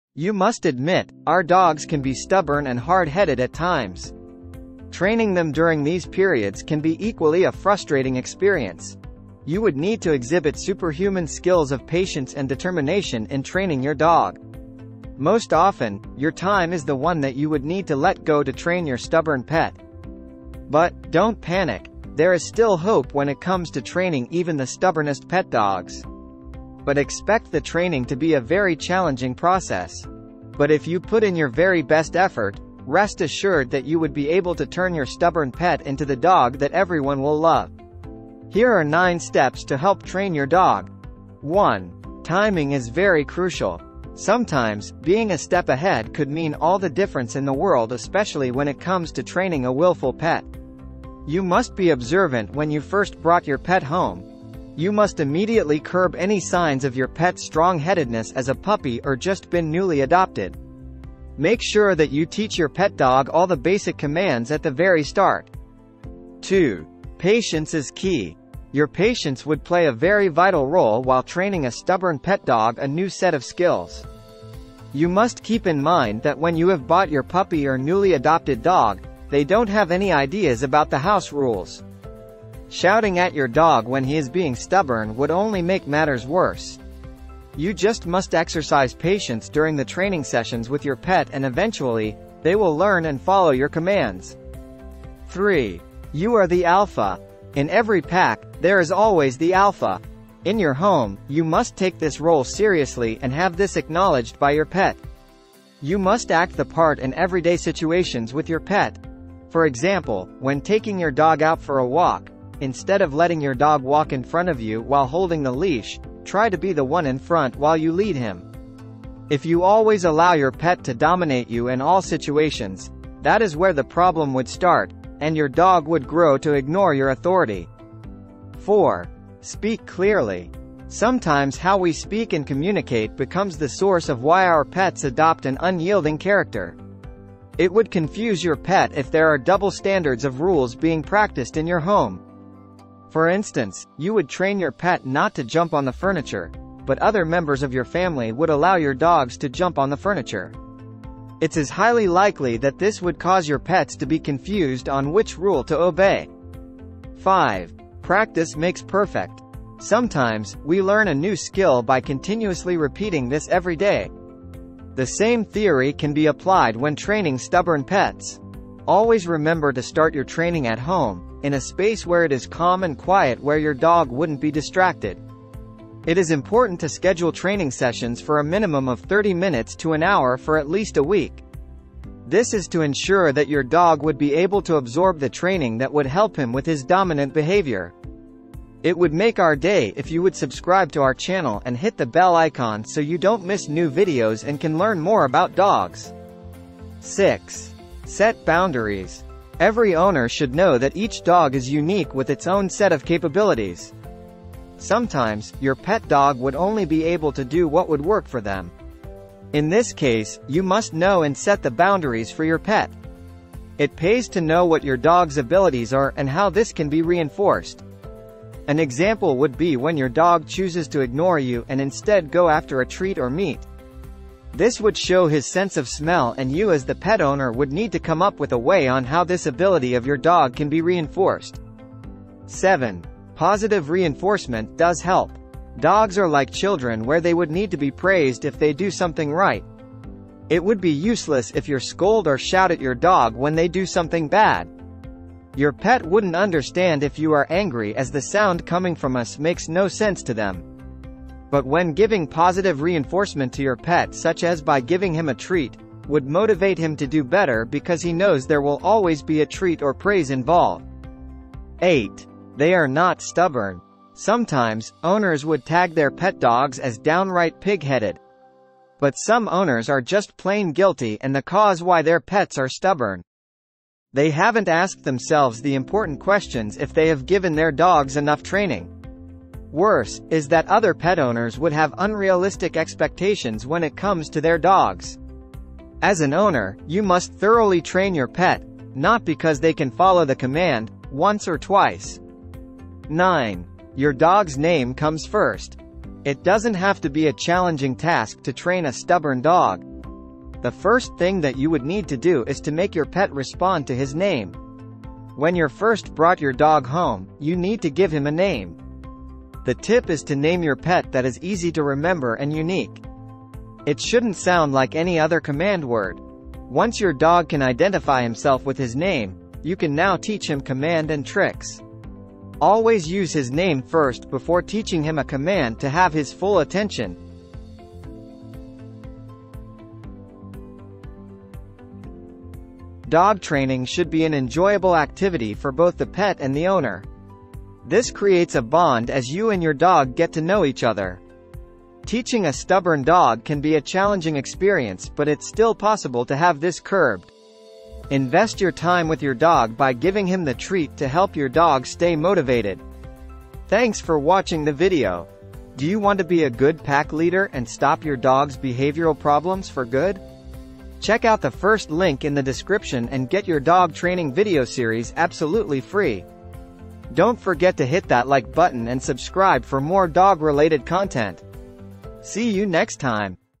How to Train a STUBBORN Puppy - Here are 9 Easy Steps!
👇 Chapters: 👇
00:00 Introduction
00:41 1. Timing is very crucial
01:06 2. Patience IS key
01:34 3. You are THE ALPHA
02:08 4. Speak clearly
02:36 5. Practice makes perfect
03:16 6. Set Boundaries
03:52 7. Positive reinforcement does help
04:21 8. They are NOT stubborn
04:52 9. Your Dog’s Name Comes First
05:28 Conclusion

You must admit, our dogs can be stubborn and hard-headed at times.

Training them during these periods can be equally a frustrating experience.

You would need to exhibit superhuman skills of patience and determination in training your dog.

Most often, your time is the one that you would need to let go to train your stubborn pet.

But, don’t panic, there is STILL HOPE when it comes to training even the stubbornest pet dogs.

But expect the training to be a VERY CHALLENGING process.

But if you put in your very best effort, rest assured that you would be able to turn your stubborn pet into the dog that everyone will love.

Here are 12 steps to help train your dog.